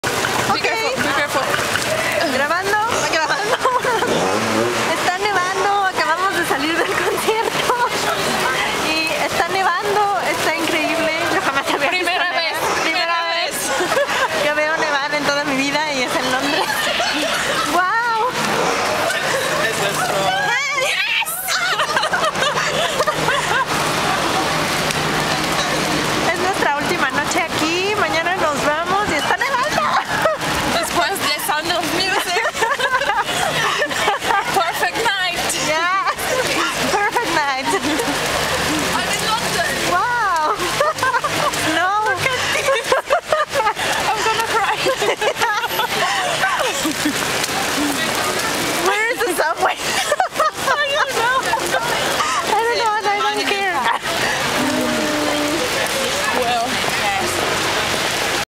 Snowing in London
Enjoying my first contact with snow ever during my trip to London in October 2008. Perfect night!